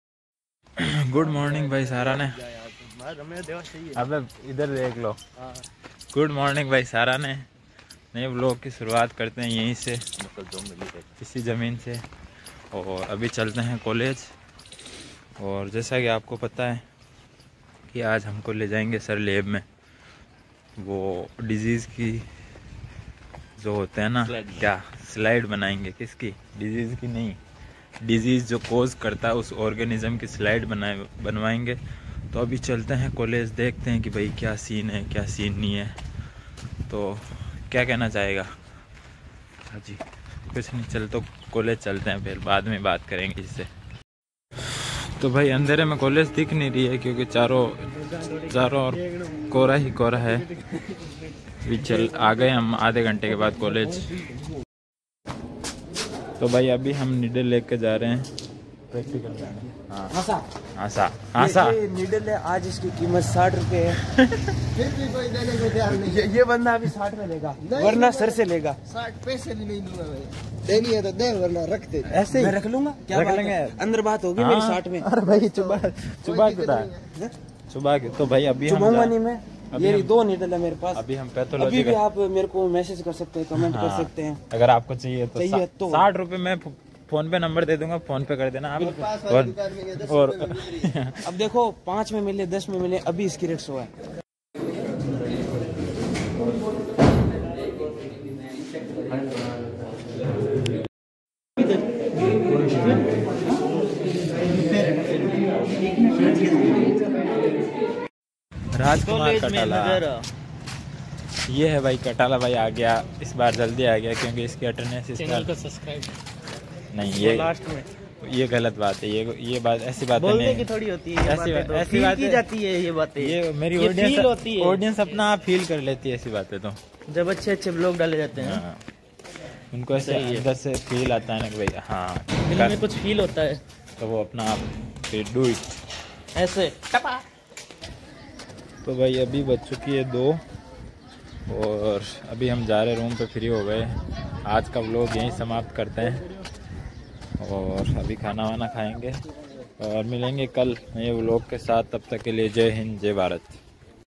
A Day in Practical Class | agriculture student की Practical class | Daily routine vlog
Title: "Chintu's Agricultural Adventure: A Day in Practical Class | College of Agriculture, Baytu Barmer"

Description:
Hey, everyone! Join me, Chintu, as I take you through a day in my life at the College of Agriculture in Baytu Barmer. 🌿 Today's video is all about our practical class – from soil testing to crop management, witness the hands-on experiences that shape our agricultural education.